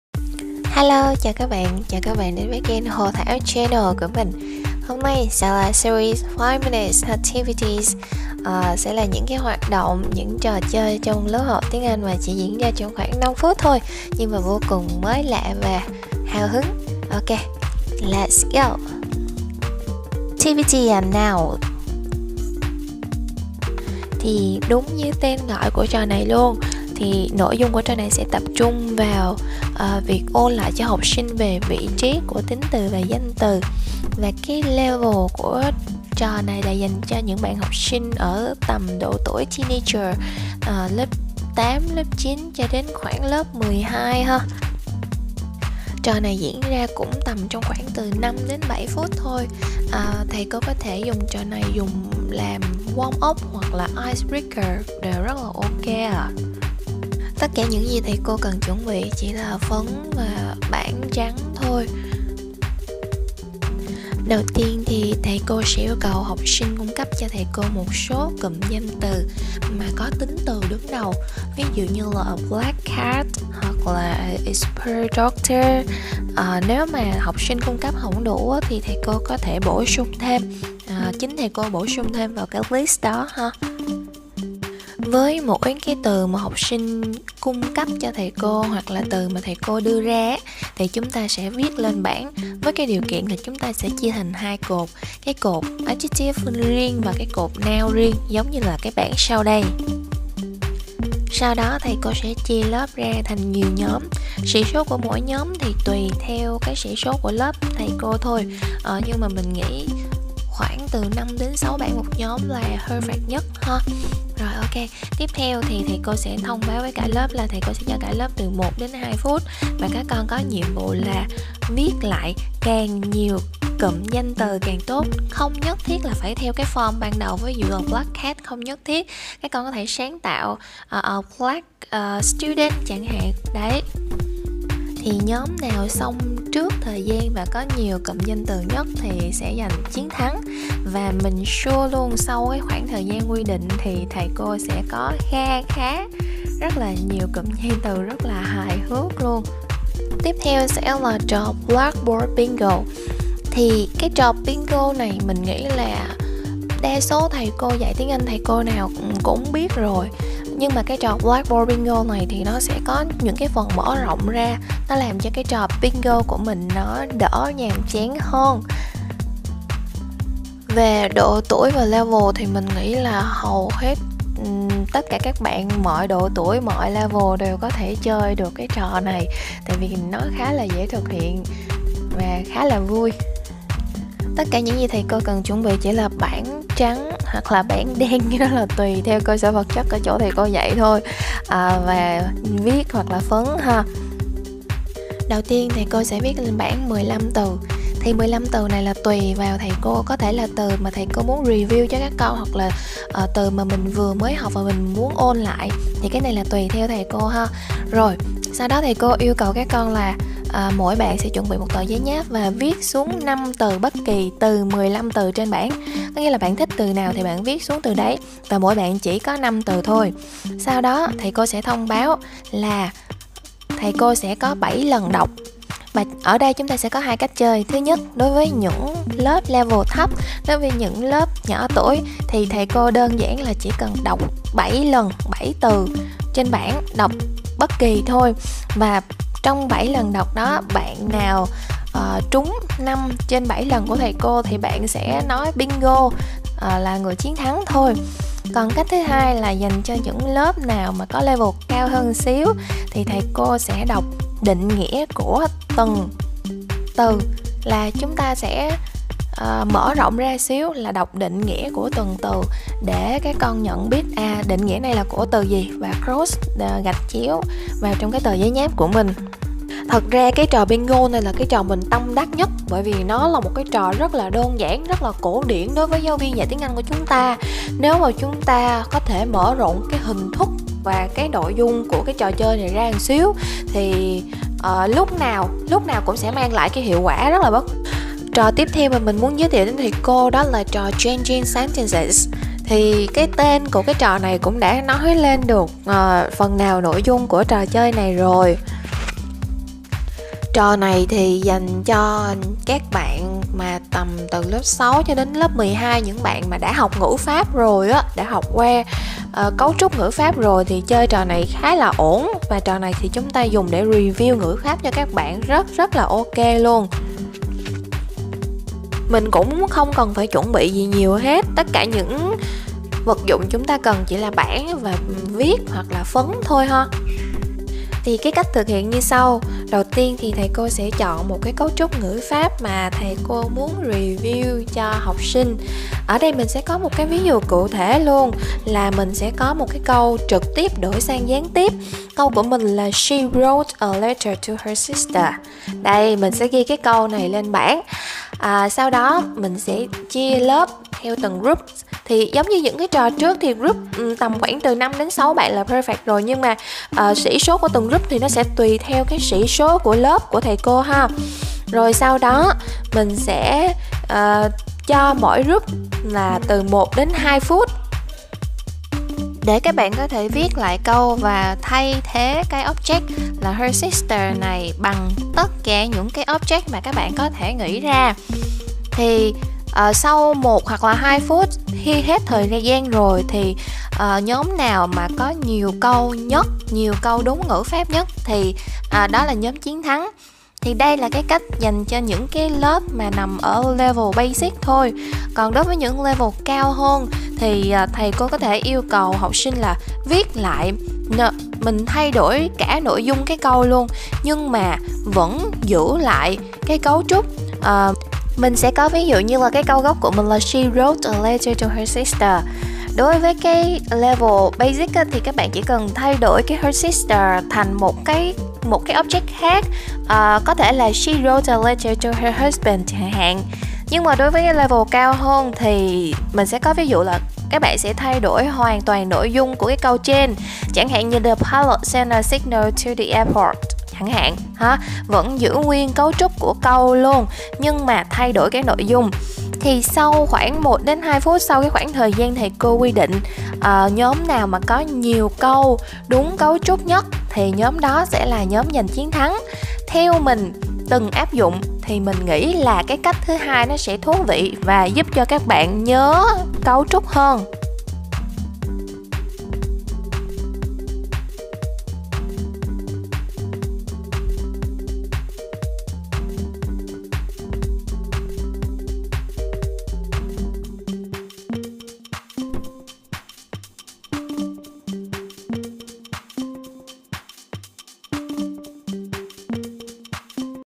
Trò Chơi Lớp Học Tiếng Anh - 5minute Activities #1
Với video Trò Chơi Lớp Học Tiếng Anh - 5minute Activities #1 thì đây  là video đầu trong series 5-minute activities của mình. hy vọng nó sè giúp thầy cô trong việc tạo nên môt tiết học Tiếng Anh thật vui và hiệu quả, tất cả nhừng kiến thức trong video đều do mình từng áp dụng và rút ra kinh  nghiệm, hy vọng thầy cô enjoy
còn rất nhiều trò chơi tại: